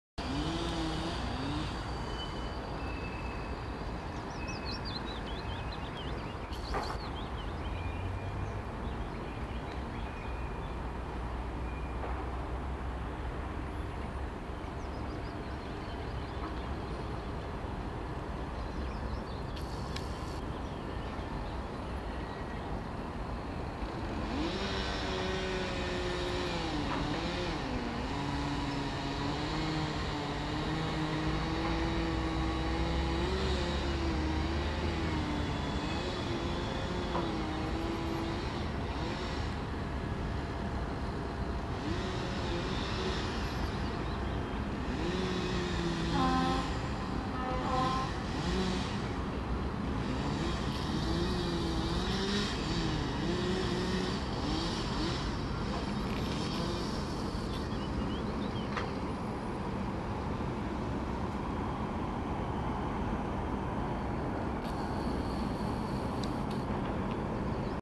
Povodeň Ústí nad Labem 10.6.2013 čas 9:30h 4 dny po kulminaci
Video natočeno z Mariánské skály dne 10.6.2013 v čase 9:30h